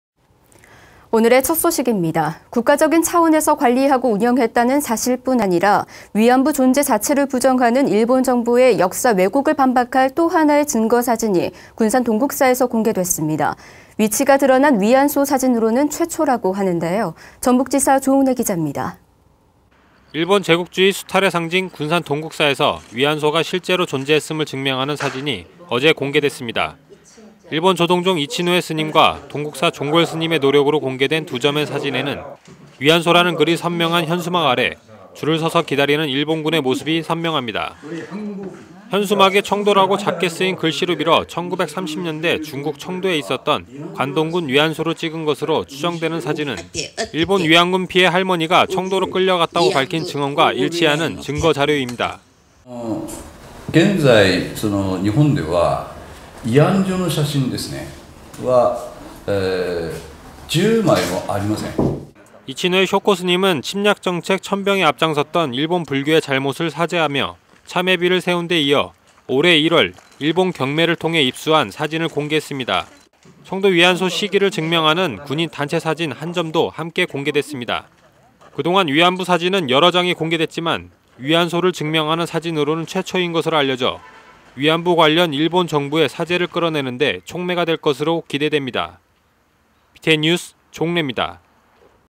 [BTN뉴스]일본군 위안소 증명할 최초의 사진 공개
국가적인 차원에서 관리하고 운영했다는 사실뿐 아니라 위안부 존재 자체를 부정하는 일본 정부의 역사왜곡을 반박할 또 하나의 증거 사진이 군산 동국.. BTN불교TV | 09-11